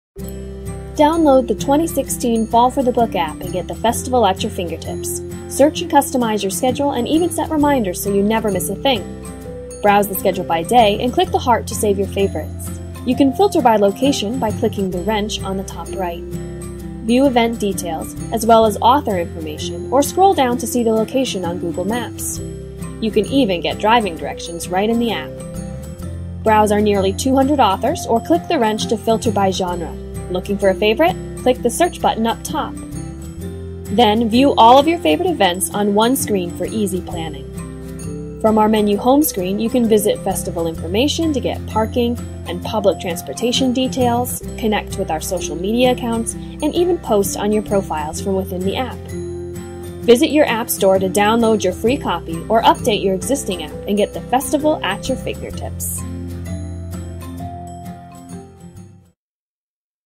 2016 Fall for the Book App Demo
Take a quick tour of Fall for the Book's 2016 app and see all the new features!